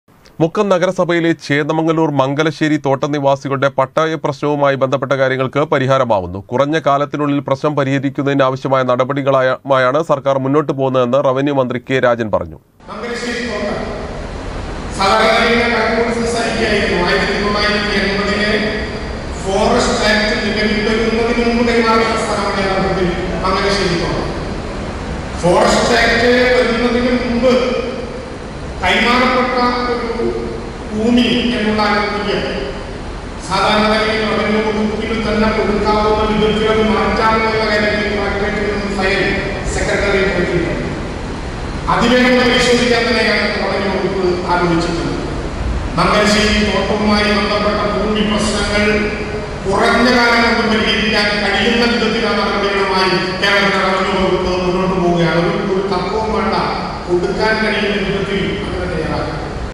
മംഗലശ്ശേരി തോട്ടം നിവാസികളുടെ പട്ടയപ്രശ്നവരുമായി ബന്ധപ്പെട്ട പ്രശ്നങ്ങൾക്ക് പരിഹാരമാവുന്നു
മുക്കം നഗരസഭയിലെ ചേന്ദമംഗല്ലൂർ